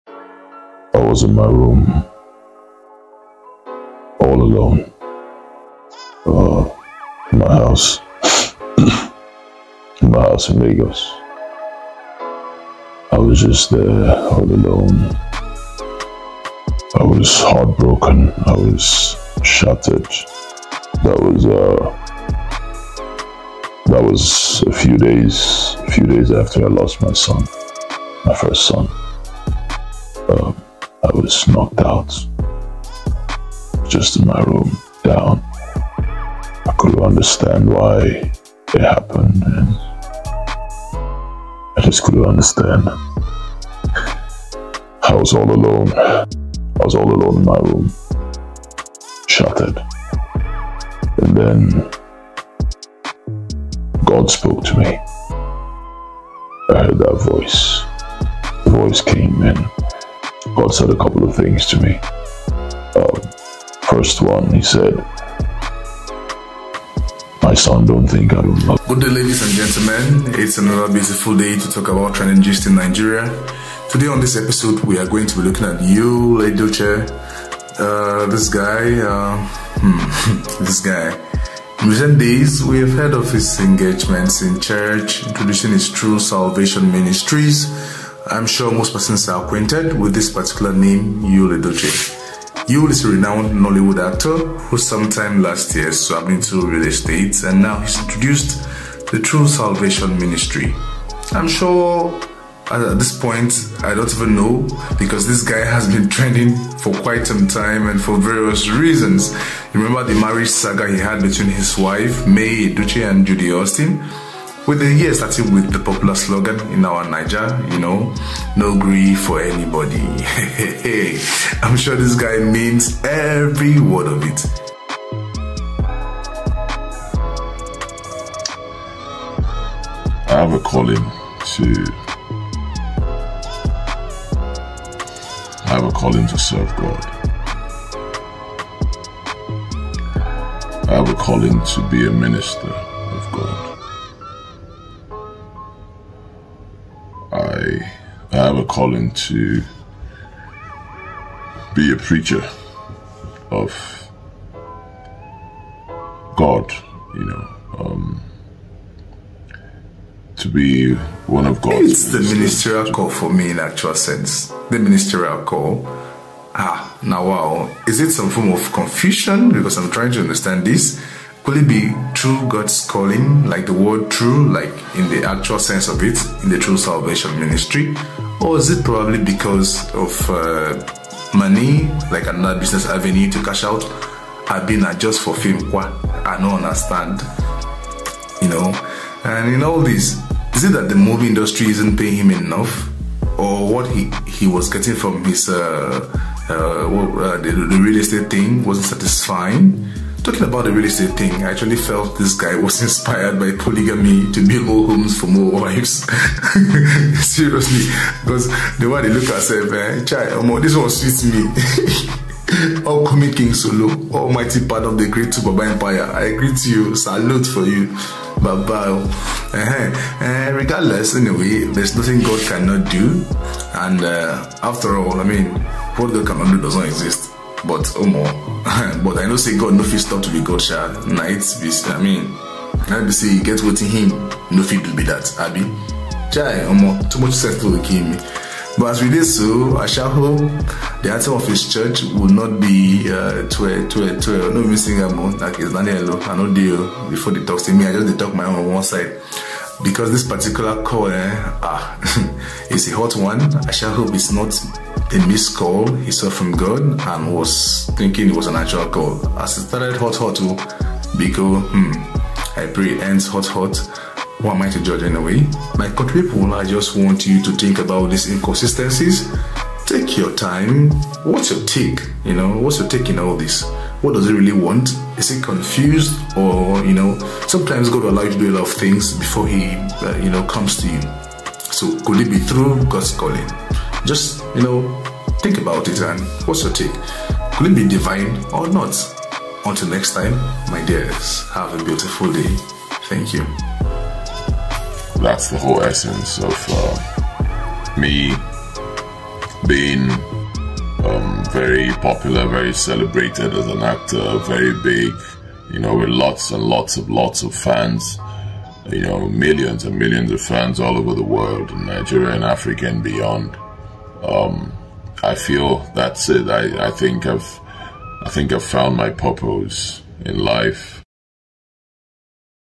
The real reason Yul Edochie became a pastor?
Yul Edochie, the popular Nollywood actor, announced in a viral video that he had become a pastor. In the video, he explained that he felt called to serve God and wanted to dedicate his life to helping others. He also said that he would be starting his own church and would use his platform to reach out to those in need. Many of his fans were surprised by the announcement, but others applauded his decision to follow his calling. Why did he make this decision to become a pastor?